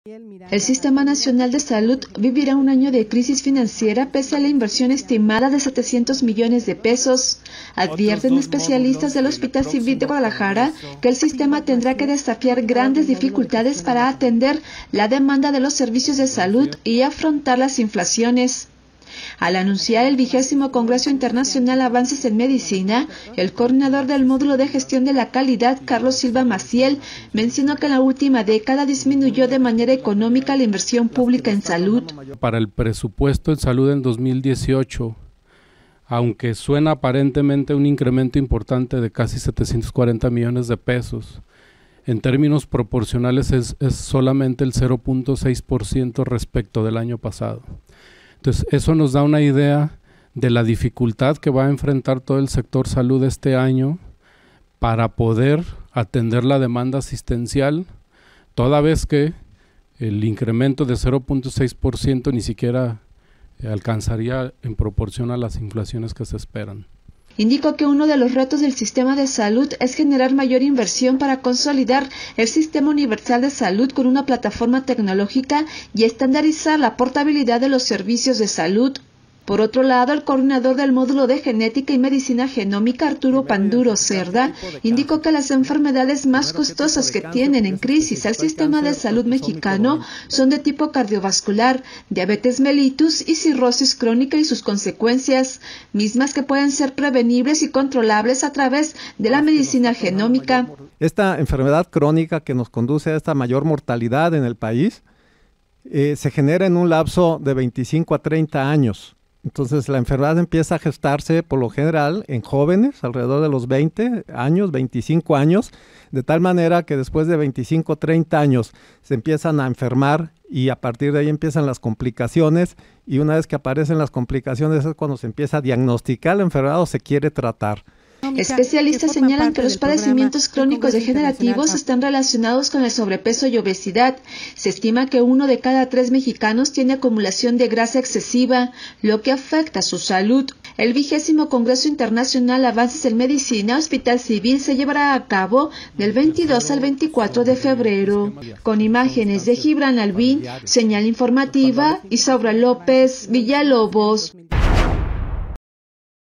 Sistema Nacional de Salud vivirá un año de crisis financiera pese a la inversión estimada
Sistema Nacional de Salud vivirá un año de crisis financiera pese a la inversión estimada de 700 millones de pesos advierten especialistas del Hospital Civil